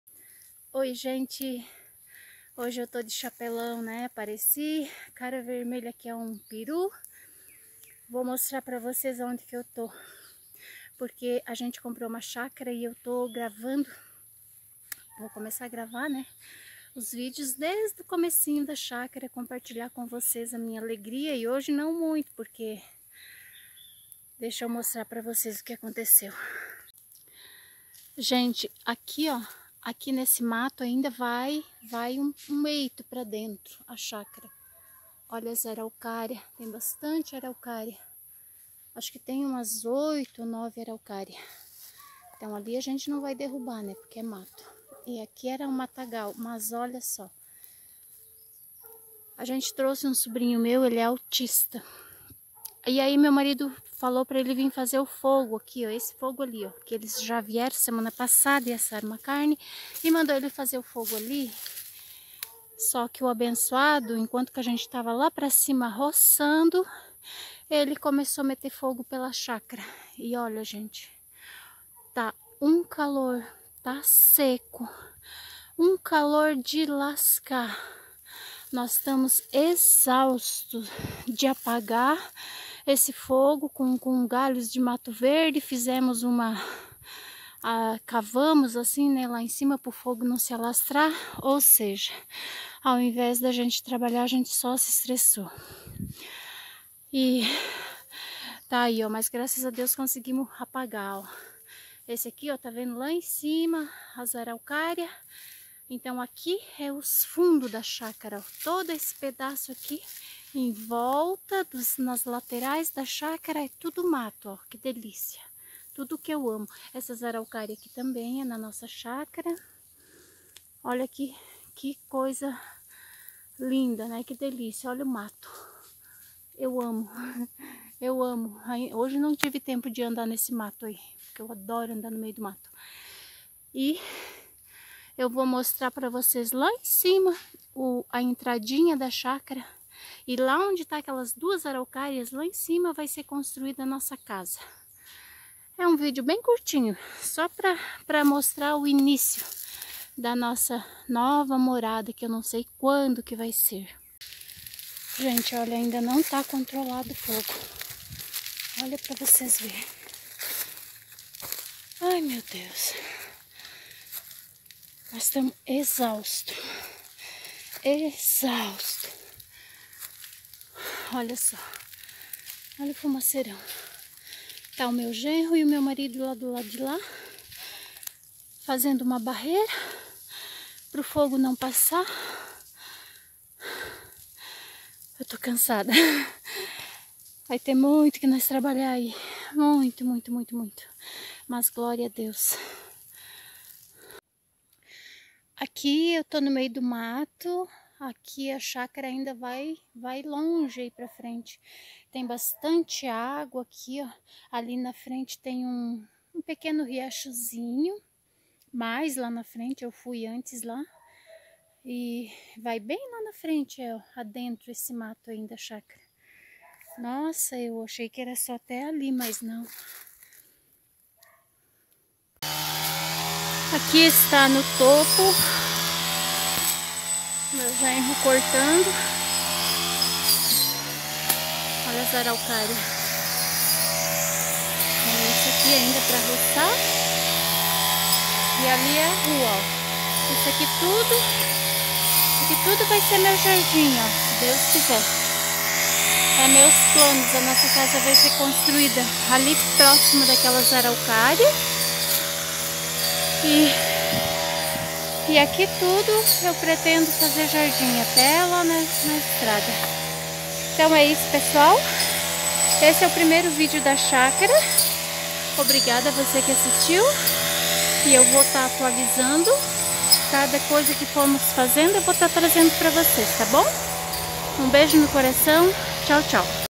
Primeiro vídeo da chácara que compramos, nossa futura morada!
07/10/19

Neste vídeo eu mostro a chácara que compramos, aonde iremos morar quando tudo estiver pronto.
Mostro um ocorrido, meu sobrinho queria ajudar, e fez varios pontos de fogo, e nós estavamos em outro lugar da chácara, só vimos quando o fogo já havia tomada uma proporção kkk deus trabalho pra apagar mas graças a Deus deu tudo certo.
Este foi o primeiro vídeo.
Vou atualizando conforme o andamento de muuuito trabalho que tem pra fazer lá.